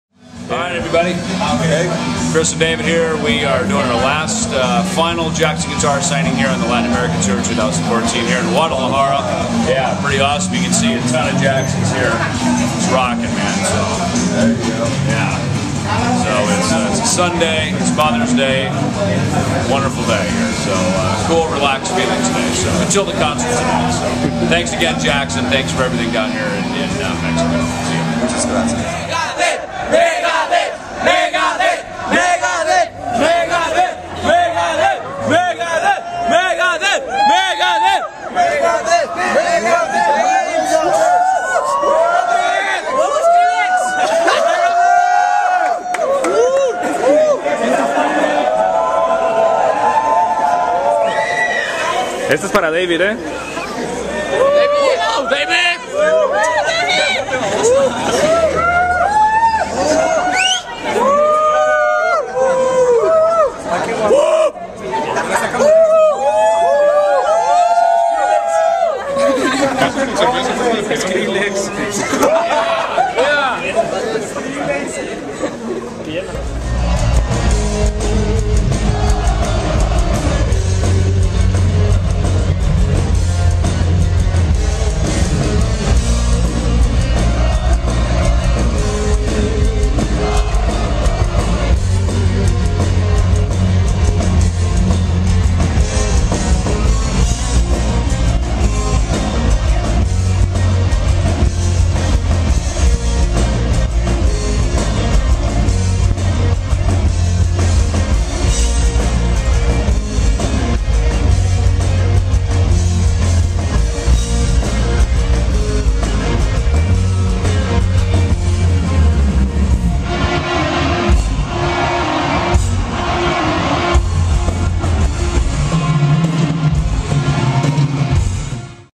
David Ellefson Chris Broderick MEGADETH Guadalajara Jackson Guitars
Ellefson & Broderick of MEGADETH appear for Jackson Guitars in Guadalajara, Mexico 2014.